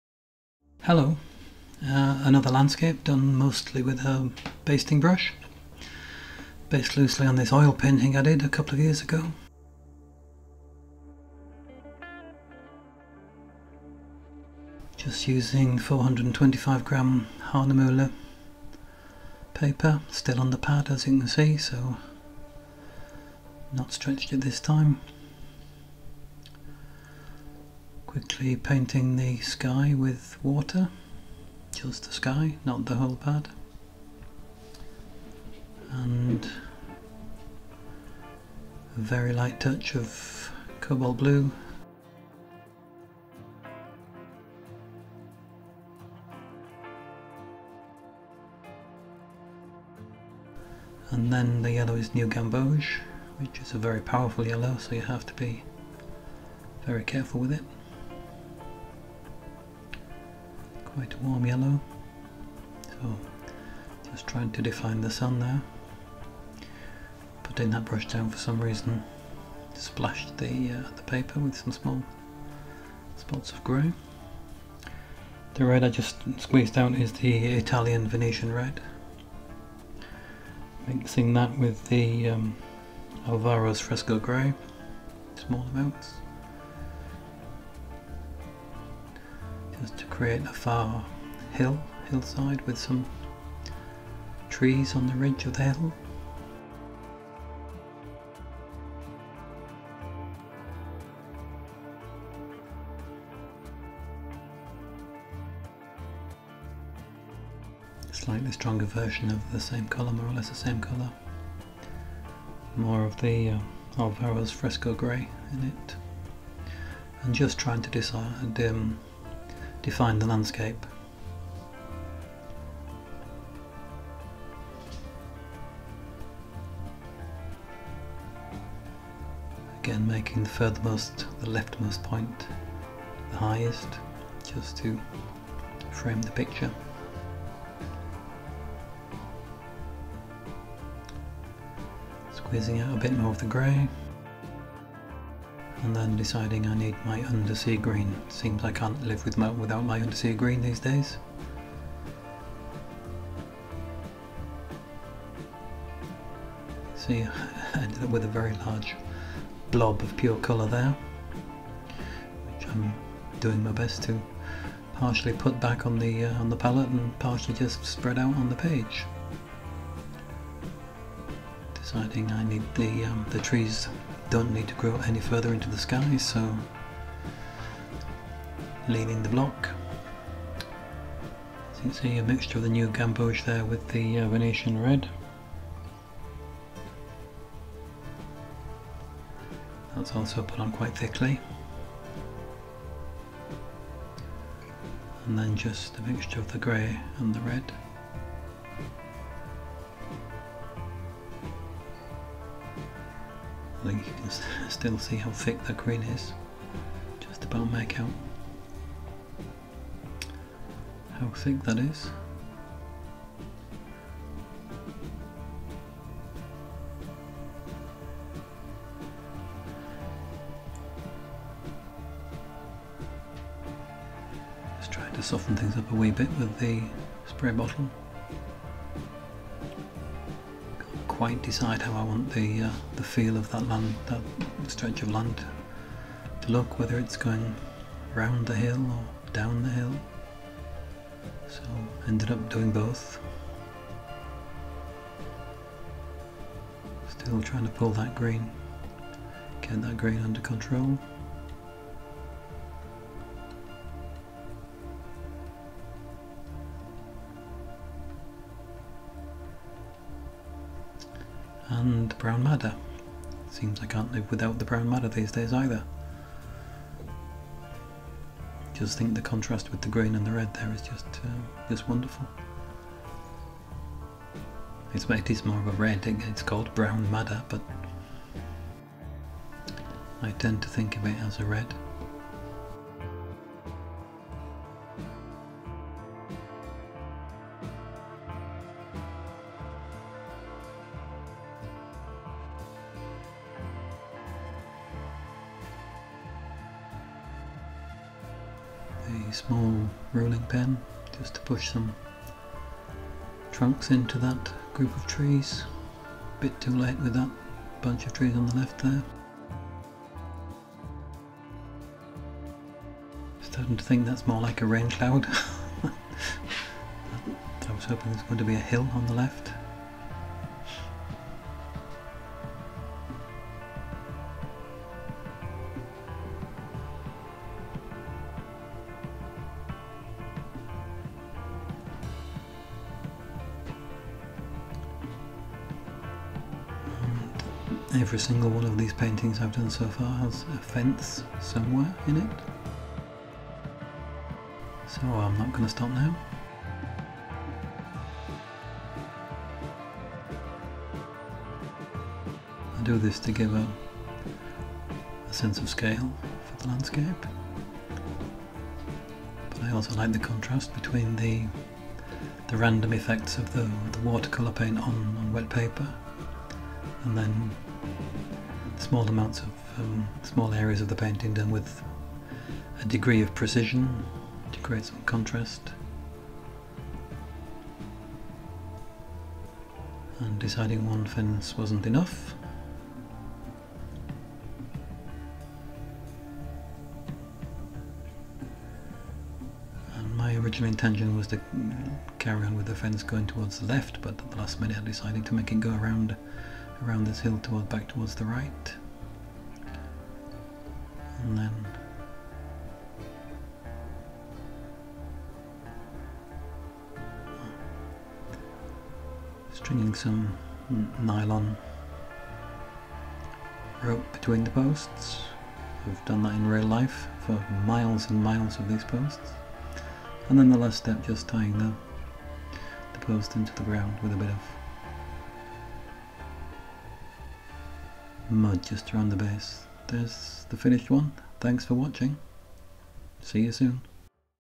Captivating Sunrise in Sibbarp: A Loose Watercolor Reimagining of my Oil Painting
Colours used:

Daniel Smith:
Alvaro's Fresco Grey
New Gamboge
Italian Venetian Red
Cobalt Blue
Undersea Green

W&N:
Brown Madder

Paper: Hahnemühle 425g